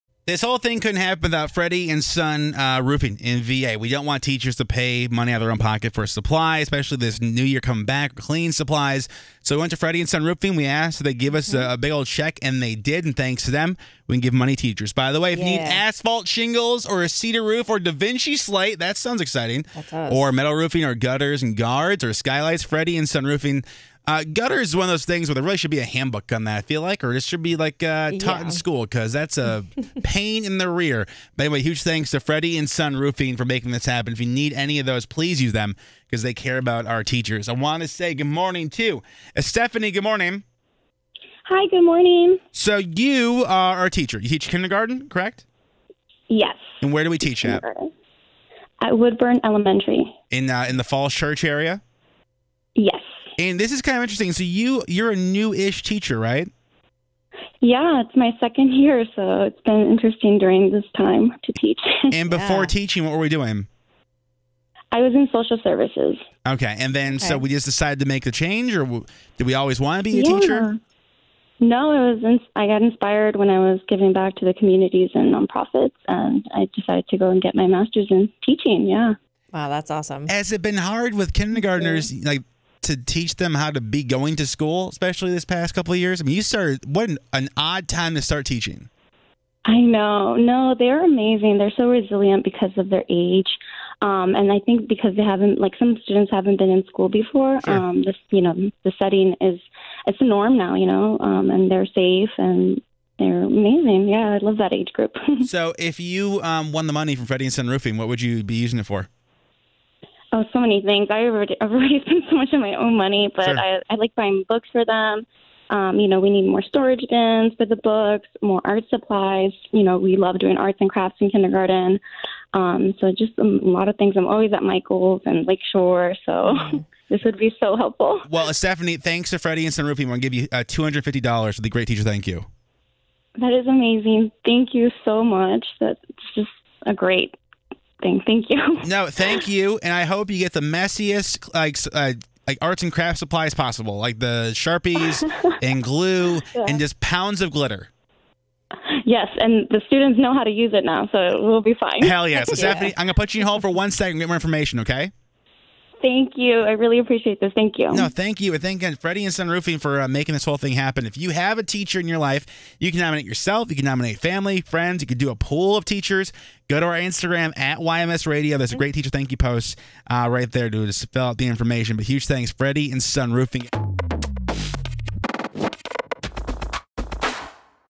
Estefani In Fairfax Is The Next Teacher To Win $250!
Thanks to Freddy & Son Roofing LLC., Estefani in Fairfax is the next teacher this year to get $250 for her classroom in our GreatTeacherThankYou!